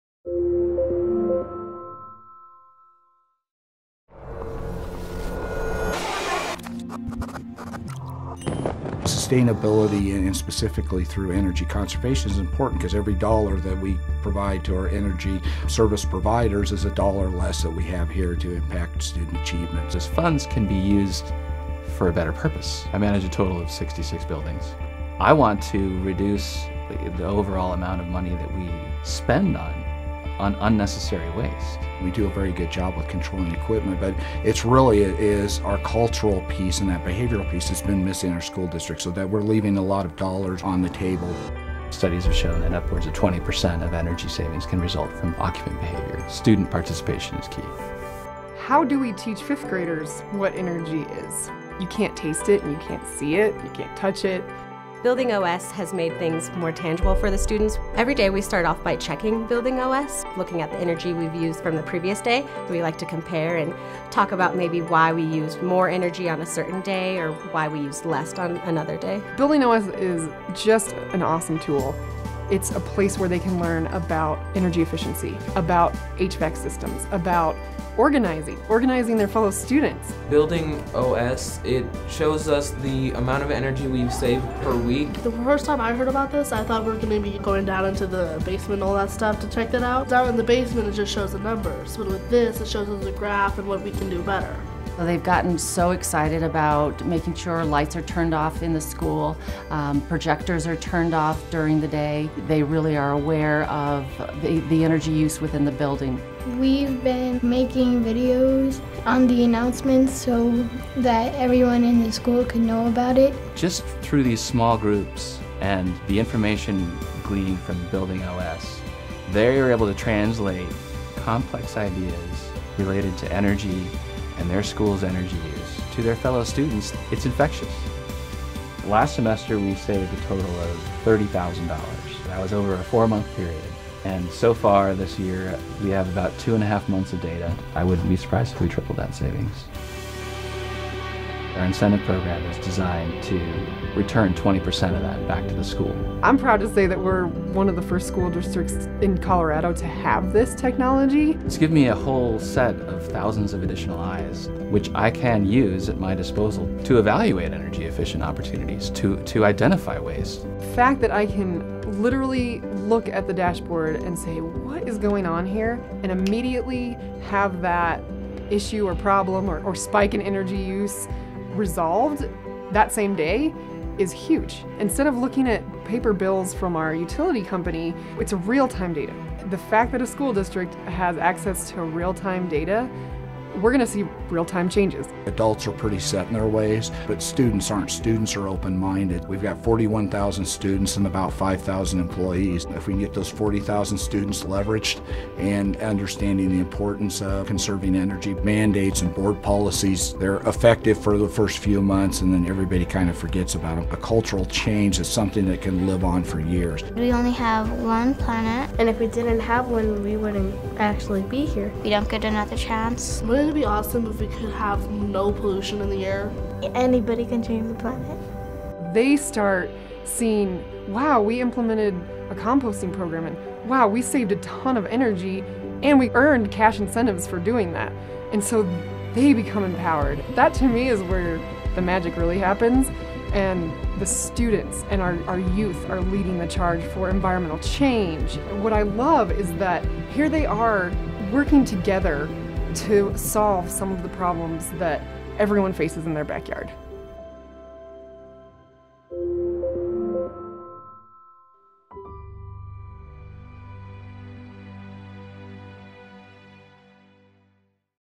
Lucid - Aurora Public Schools uses BuildingOS to Save Energy and Engage Students
Learn how Aurora Public Schools engage their students using their own energy use data, and how they are saving thousands of dollars in energy costs through operational and behavioral changes.